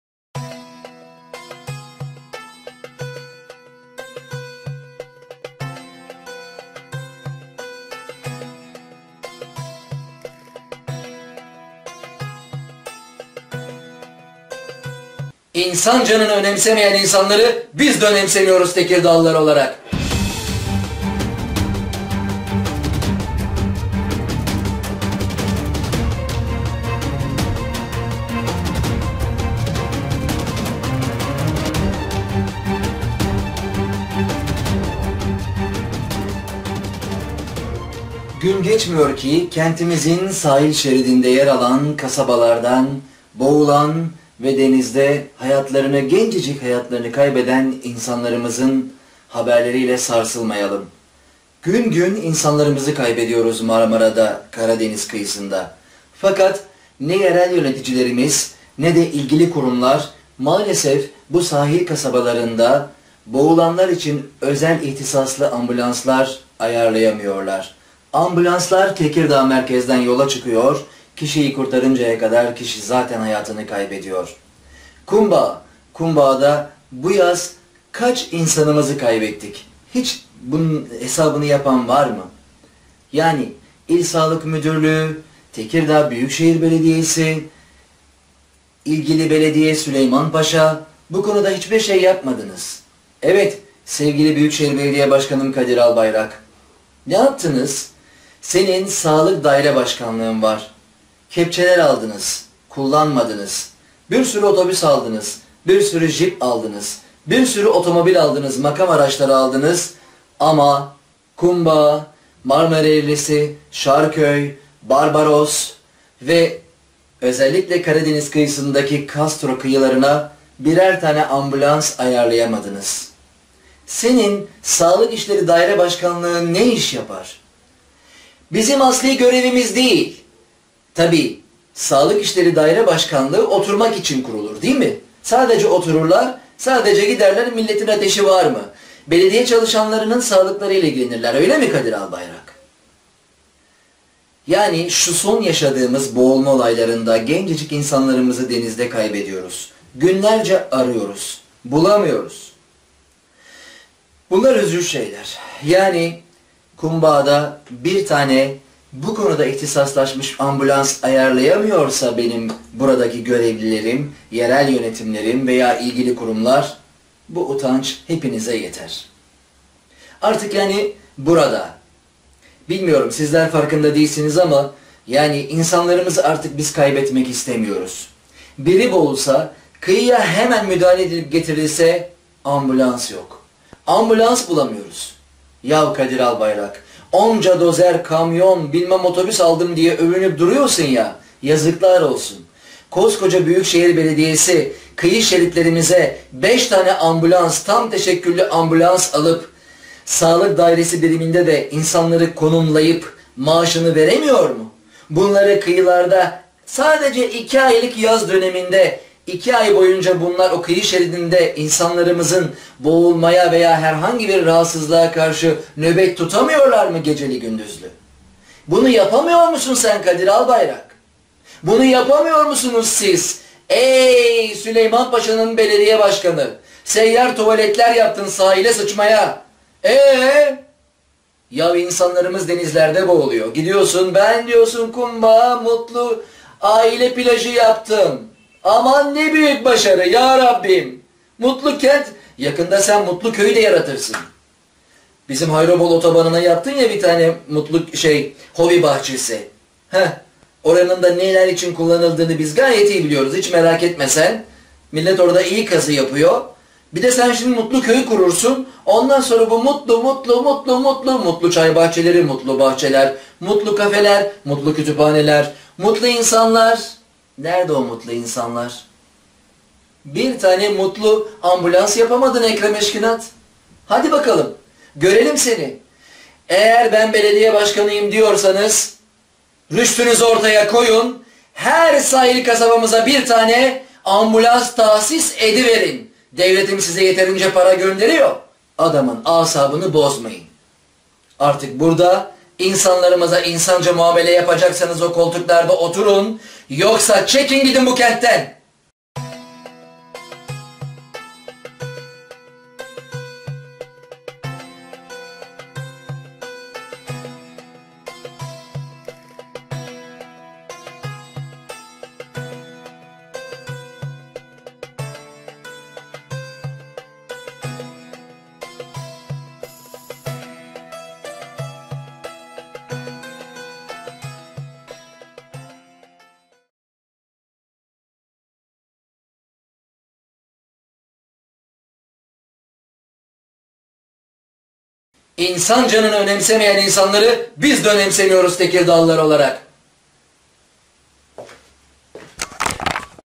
CAN KURTARAN İSTİYORUZ
Tekirdağımızın 2 aylık yaz sezonunda onlarca her yaştan insanımızı boğularak kaybediyoruz. Ambulans Merkezden yola çıkıp özellikle Kumbağ'a varması yarım saat. Oysa denizde boğulan için dakikaların hatta saniyelerin önemi var. CANLARI KURTARIN İSTİYORUM. Çok şey mi istiyorum?

Nijat Ayvaz
Lambadaki Cin
Gazeteci-Araştırmacı Yazar-Sanatçı